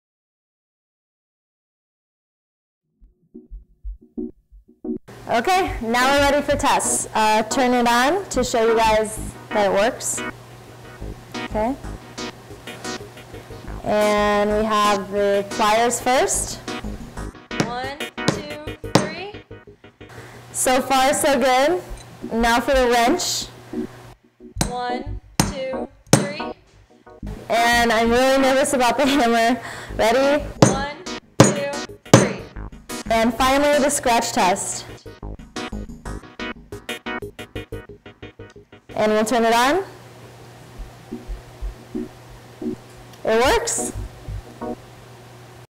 NEON FILM Scratch and Shock Test with Hammer, Driver, Pincers
Neon Film Special Properties:
1. Shock Absorption, Super High Hardness (~9H) High Quality Cell Phone Protection film, 2. Super Easy to Attach without Bubbles
3. Anti-scratches can withstand damage from Pincers, Hammers, Drivers
4. One Neon film protect your screen from Costly repair of USD$ 200 to USD$ 250.